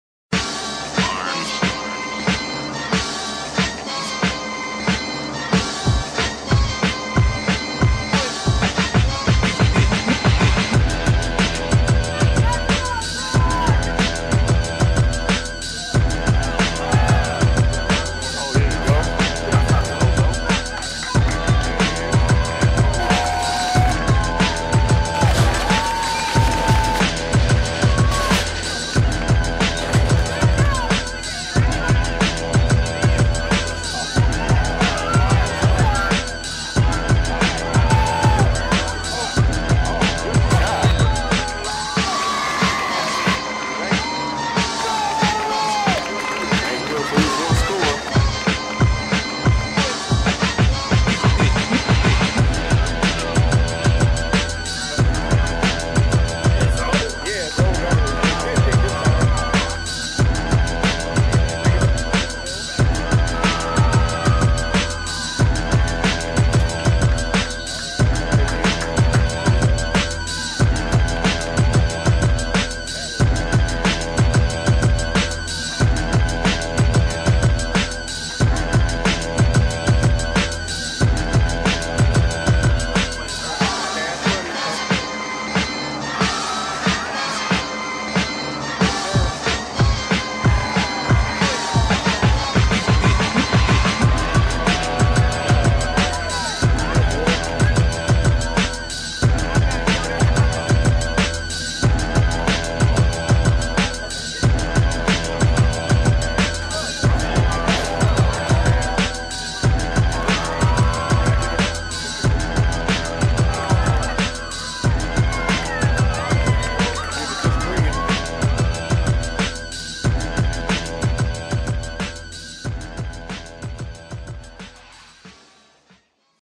Finney vs Henry Ford 2008- Top 5 Plays of the Week
These are the Top 5 Plays of the Week for Detroit Henry Ford, as they host Detroit Finney on 10/3/2008.